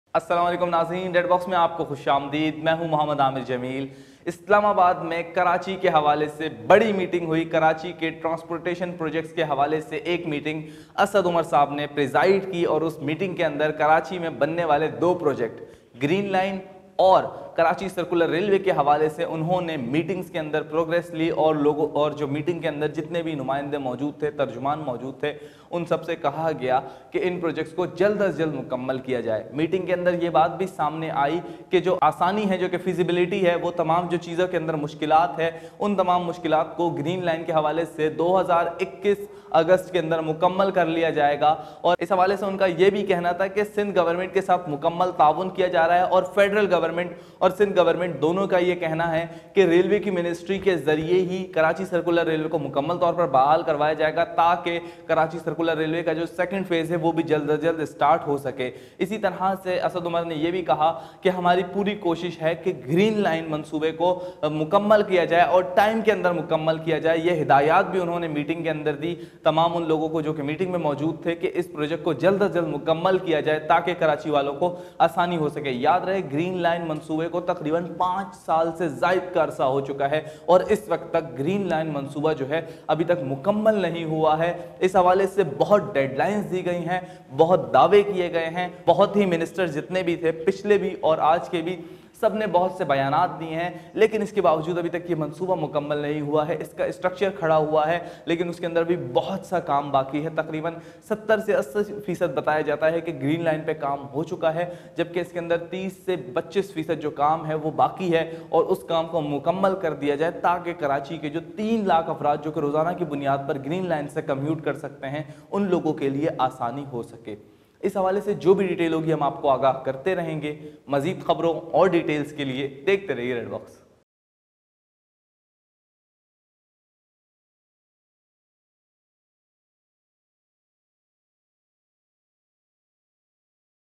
NEWS UPDATE : GREEN LINE BUS KARACHI | KCR PROJECT | TRANSPORT SYSTEM | SINDH | FEDERAL GOVT. | RBTV
The feasibility study of the Karachi Circular Railway project (KCR) is likely to be completed by August 2021 while the project’s bidding and financial close are also expected to be completed within the year.
Federal Minister for Planning, Development, and Special Initiatives Asad Umar chaired a meeting to review the pace of the Green Line BRT and KCR projects here in Islamabad on Wednesday. Secretary Railways, CEO PPP Authority, SIDCL, DG Planning Railways, and senior officials participated in the meeting.